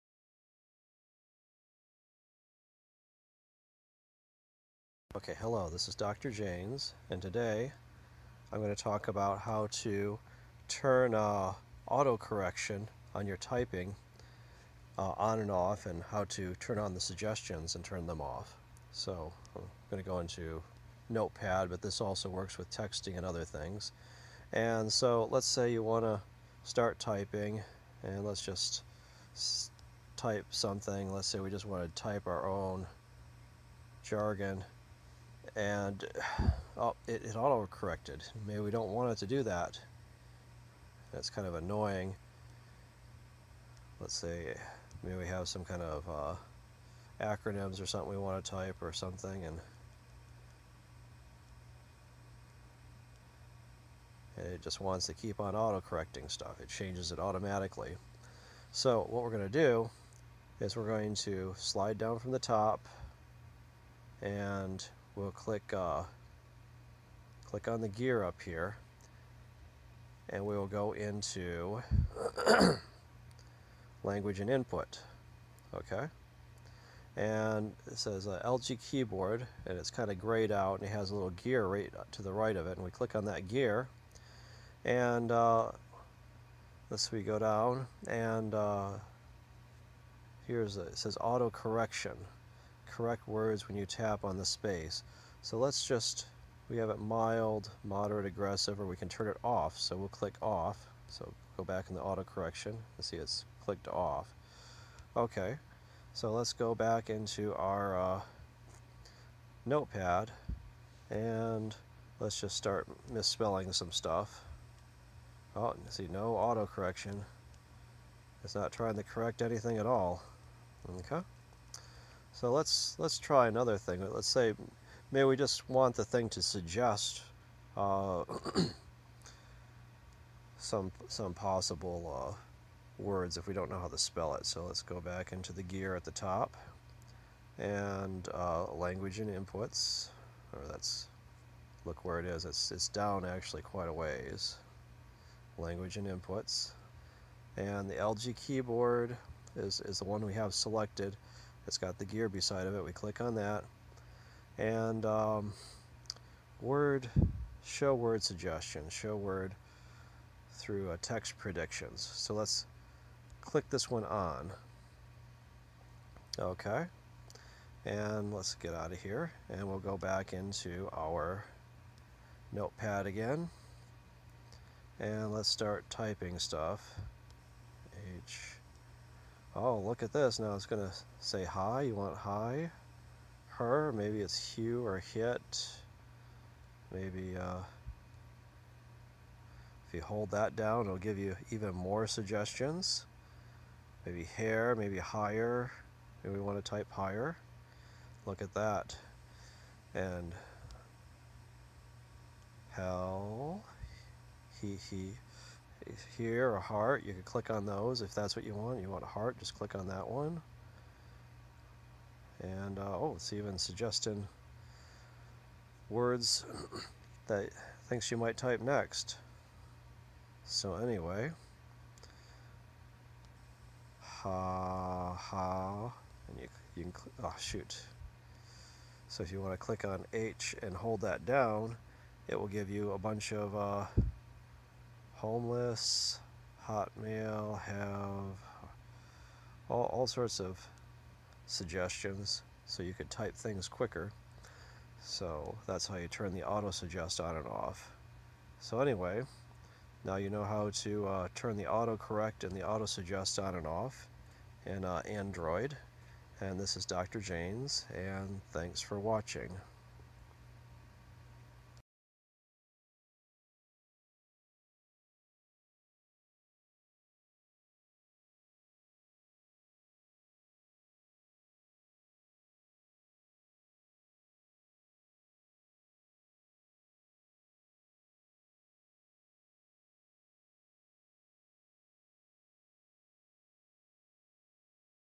Android: How to Change Text Autocorrect Settings
Have you ever been texting or writing memos on your Android phone or tablet, and the stupid thing keeps changing what you are writing.  You back space and try to fix it, but it changes it again?  Maybe you just want to have the device show you word suggestion so you can type and text faster by just tapping on the words in the box? This Video show you step by step how to do this and fix these problems and more...